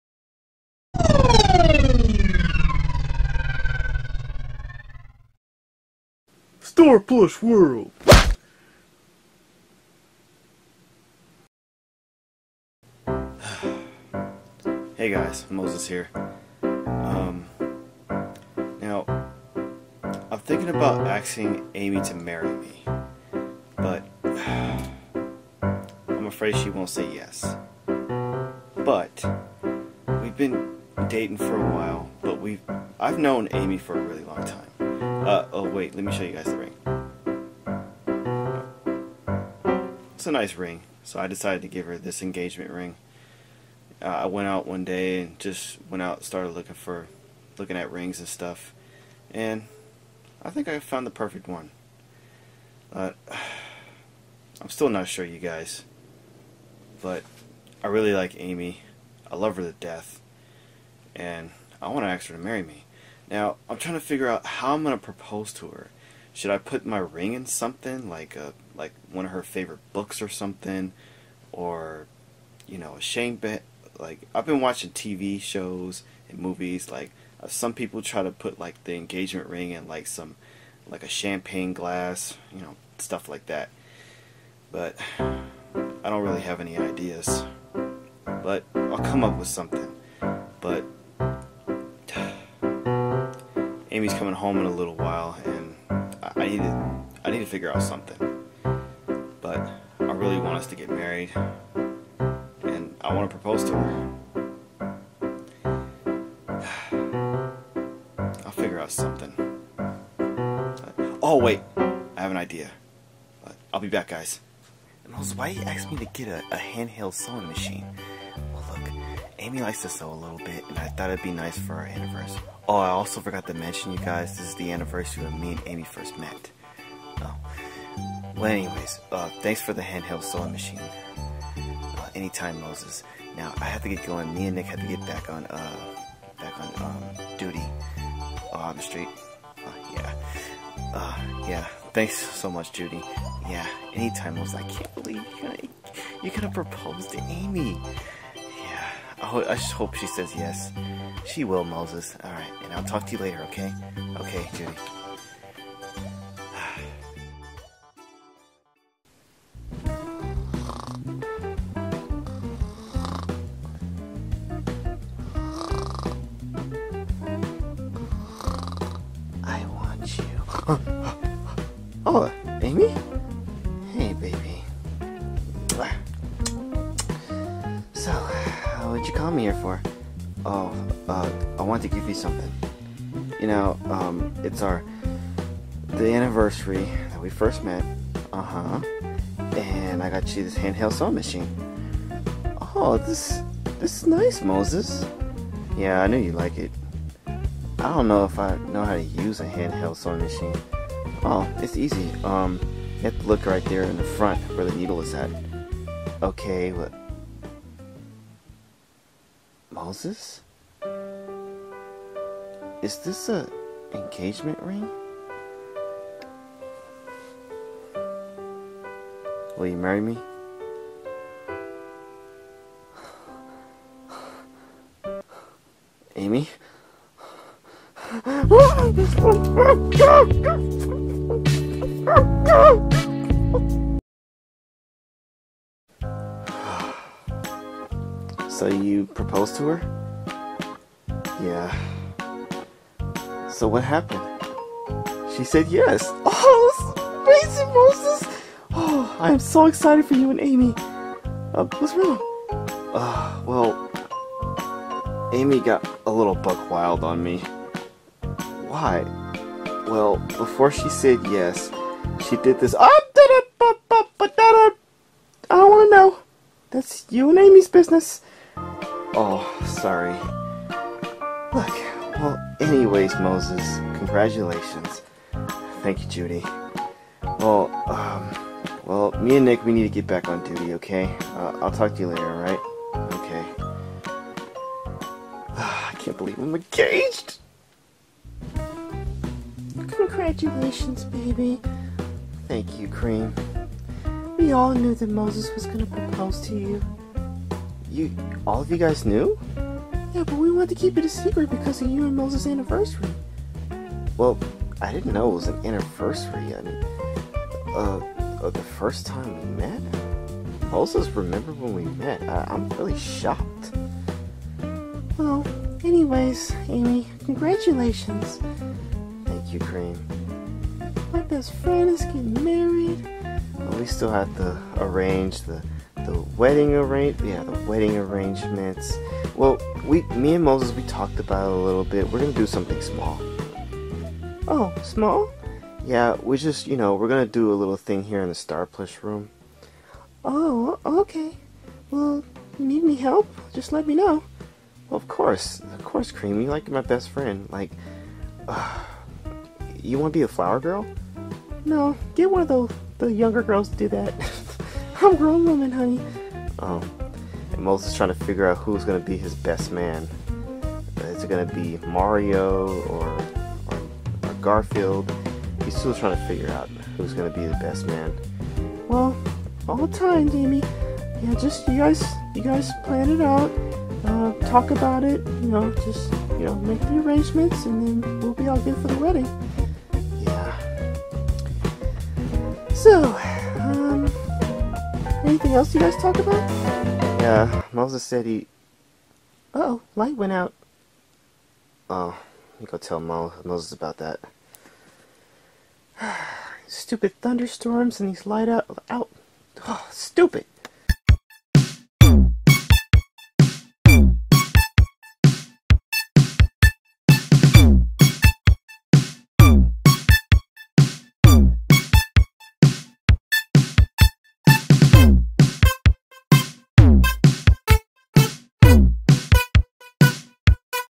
SPW MOVIE: MOSES' PROPOSAL TO AMY ROSE!!!
Hi guys. whew. today, I'm going to propose to Amy; I found her a really nice engagement ring. If you guys don't know, me and Amy have been dating for a while; but we've known each other for a really long time. I'm hoping she says yes. I have my friend Judy Hopps helping me find a anniversary gift for Amy; it's the anniversary of when we first met by the way. I hope she says yes.